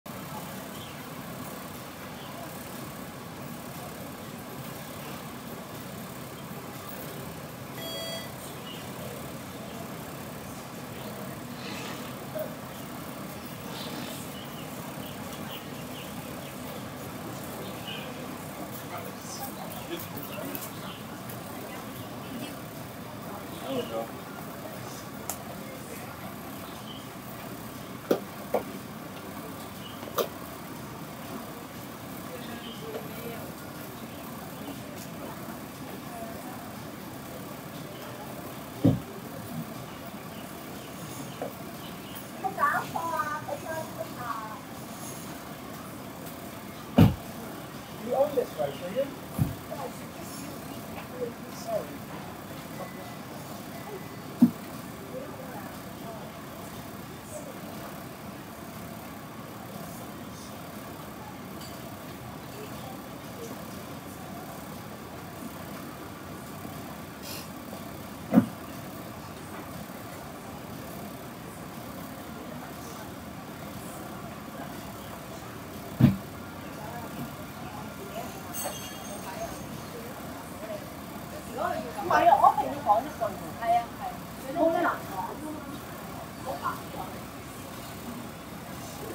直擊 - 人肉霸位已經唔啱喇，敗北仲問人有冇搞錯 😂
網友提供